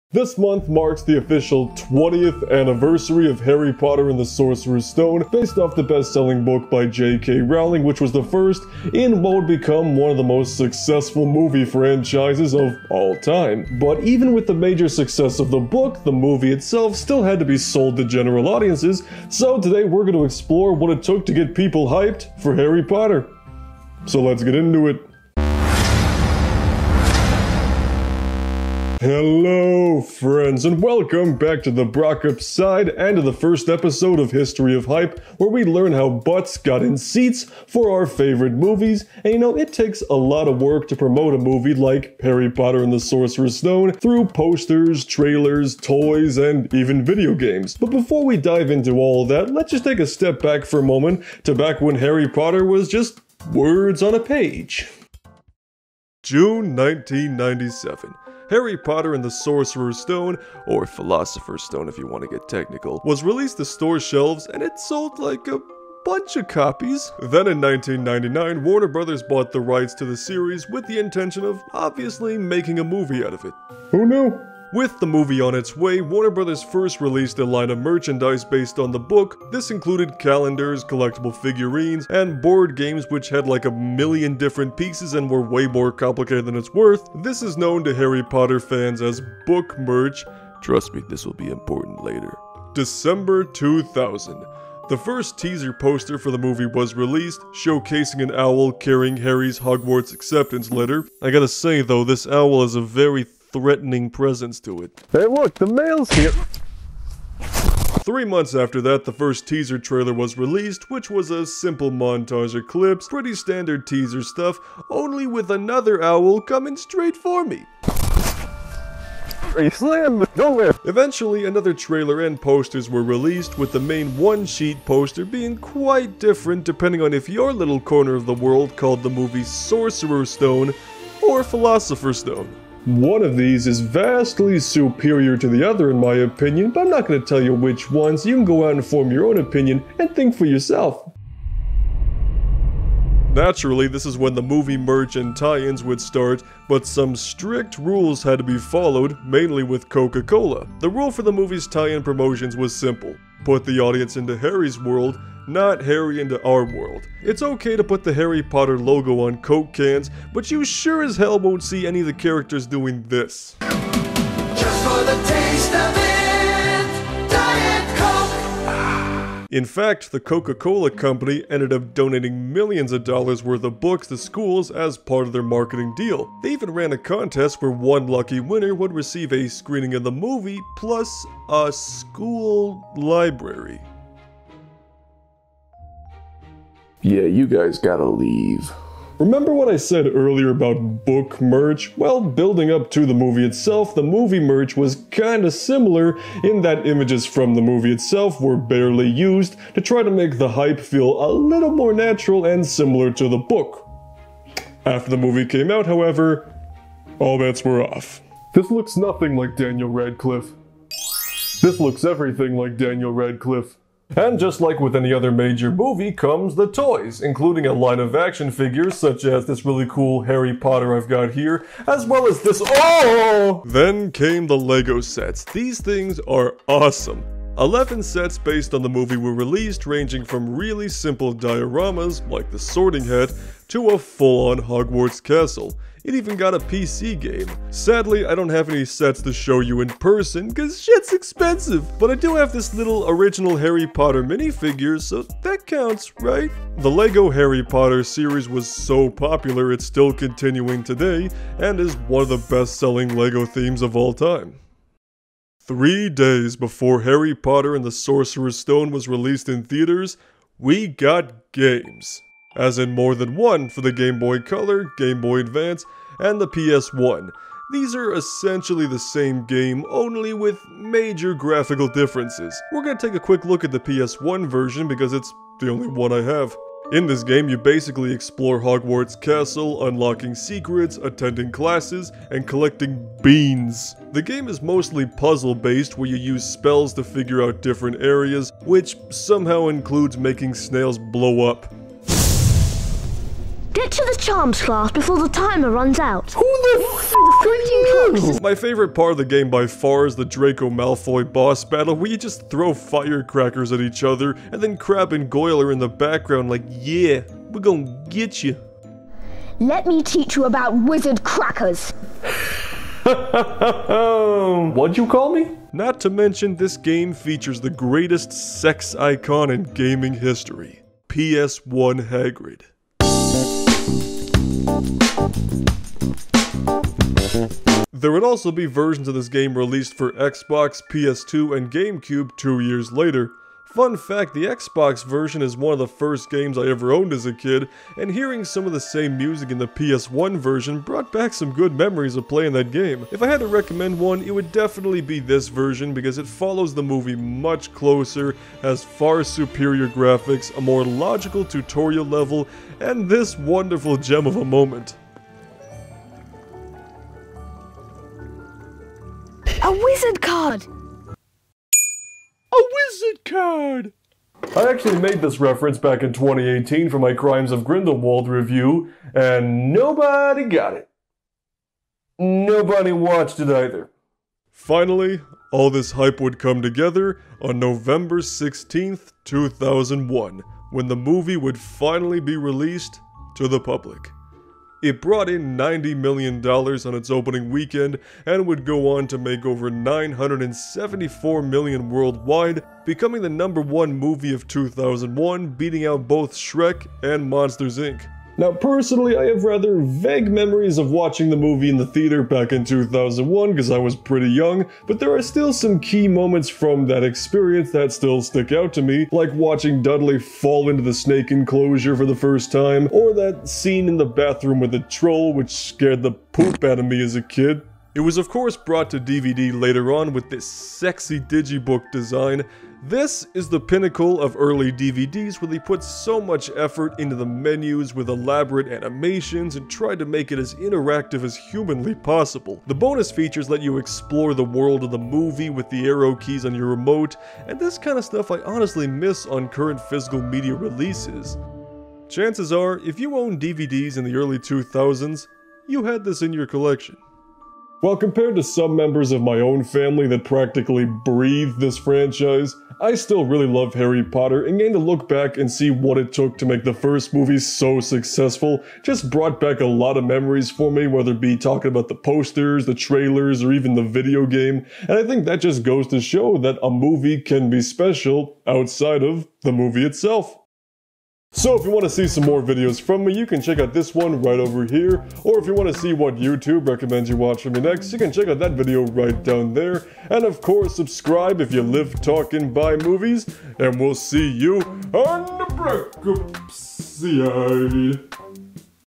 How Harry Potter Became a MASSIVE Success
This month marks the 20th anniversary of Harry Potter and the Sorcerer's Stone's movie adaptation, so let's look back and see what it took get people hyped for it.

The Brockopp Side - Live. Talk. Buy. Movies